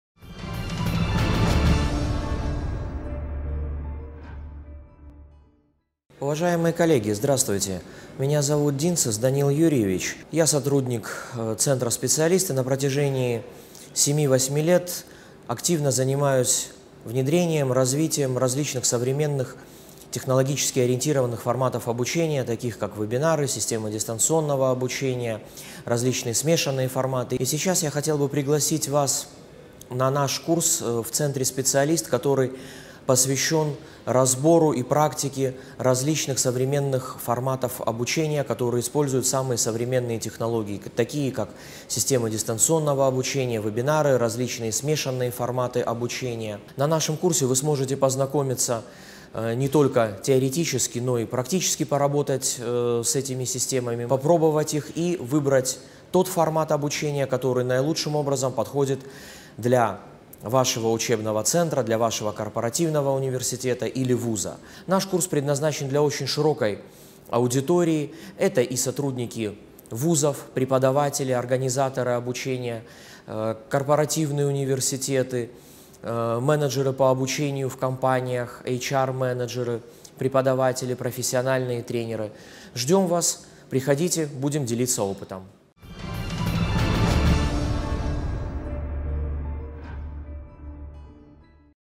Виртуальные форматы обучения: методики и технологии
Современное информационное общество активно осваивает виртуальное пространство: общается, ищет нужную информацию, получает образование в Интернете. Обучающие технологии подстраиваются под высокий темп нашей жизни, предлагая наиболее современные и удобные форматы обучения.

Чтобы обучать эффективно, необходимо идти в ногу со временем. Уникальный авторский курс расскажет Вам о лучших современных практиках центра «Специалист». Откройте для себя ноу-хау крупнейшего учебного центра России по информационным технологиям!